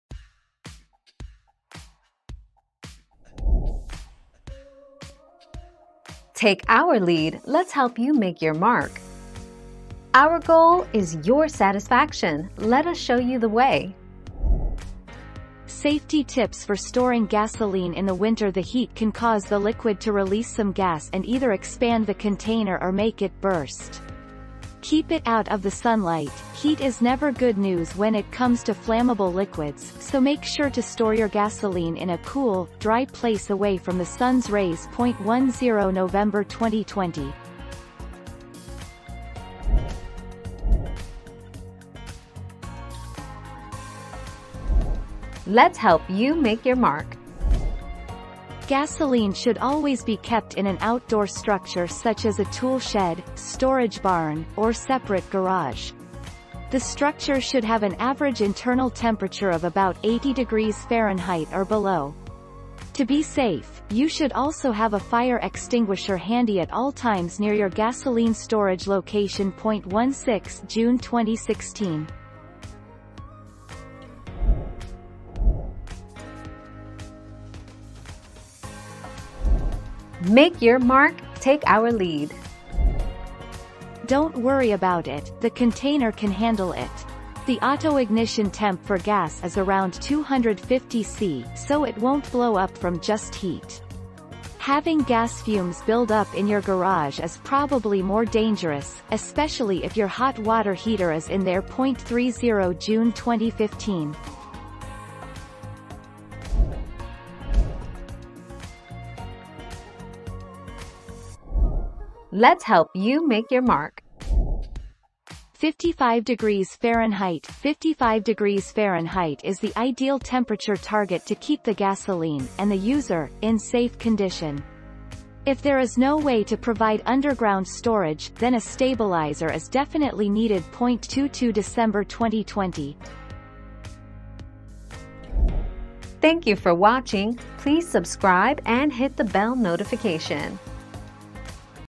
Can you store gasoline outside in the winter?
00:00 - Can you store gasoline outside in the winter?
00:41 - Is it OK to store gasoline outside?
01:16 - Is it safe to store gasoline in a hot garage?
01:48 - What temperature can you store gasoline?

Laura S. Harris (2021, March 4.) Can you store gasoline outside in the winter?
    AskAbout.video/articles/Can-you-store-gasoline-outside-in-the-winter-227332